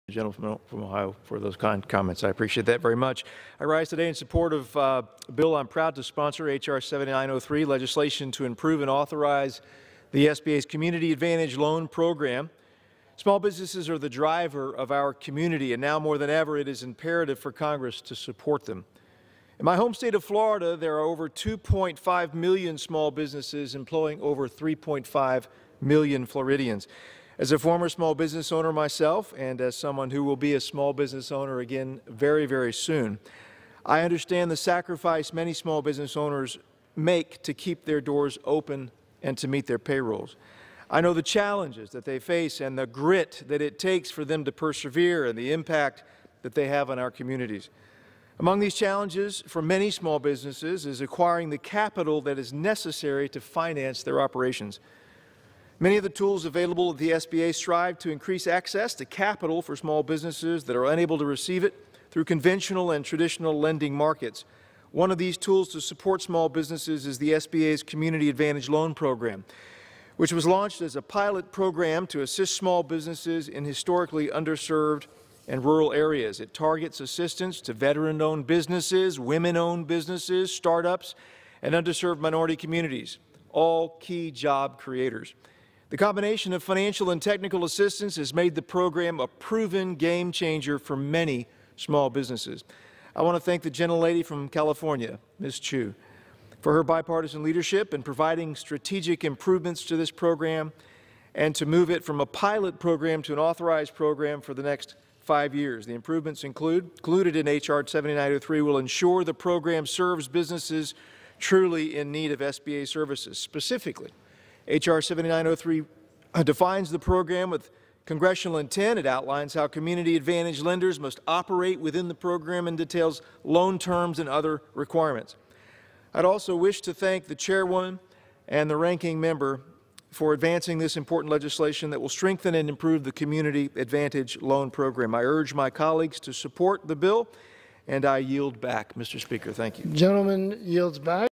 Rep. Spano on the Small Business Administration Loan Program
U.S. Representative Ross Spano (FL-15) on the U.S. House Floor discussing the benefits of, H.R. 7903, the Small Business Administration Loan Program. This bill will improve and authorize the Small Business Administration's community loan program.